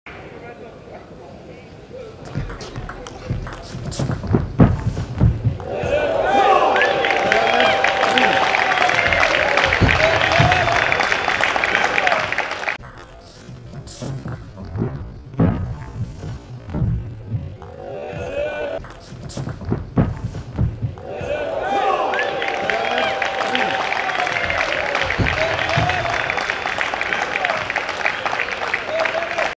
Impossible Around Net shot by Mima Ito at WJTTC 2016
Watch the best table tennis matchs on your smartphone by download TableTennisRulez official app(totally free):